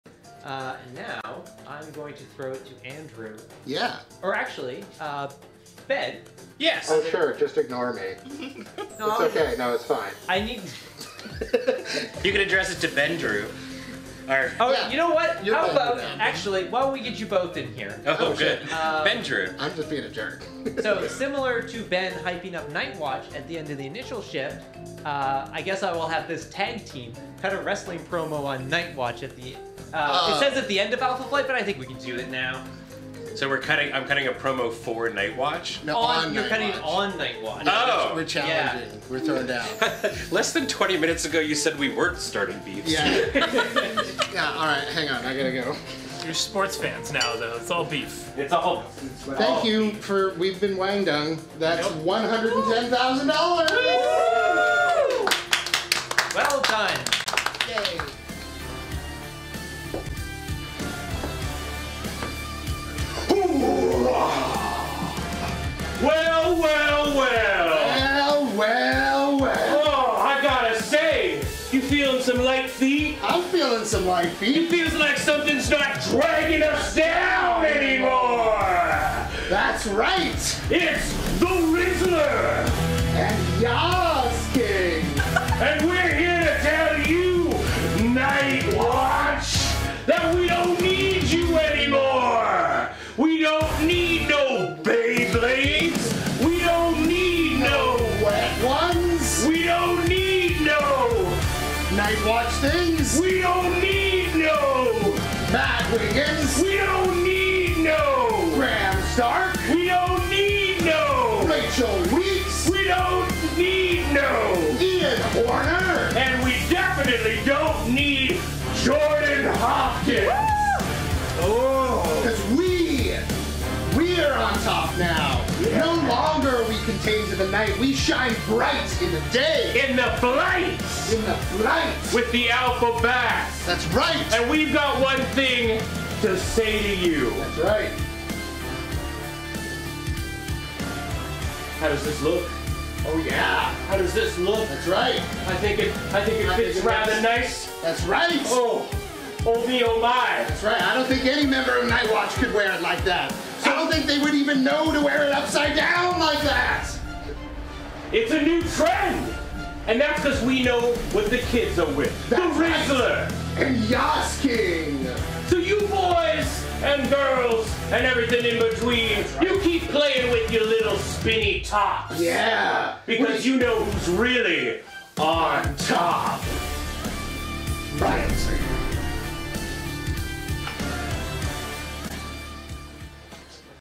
DB2024 - Ben & Andrew cut a promo on Night Watch
Ben and Andrew are challenged to cut a promo on Night Watch
00:00 - The challenge is issued and accepted
00:57 - They Don't Need No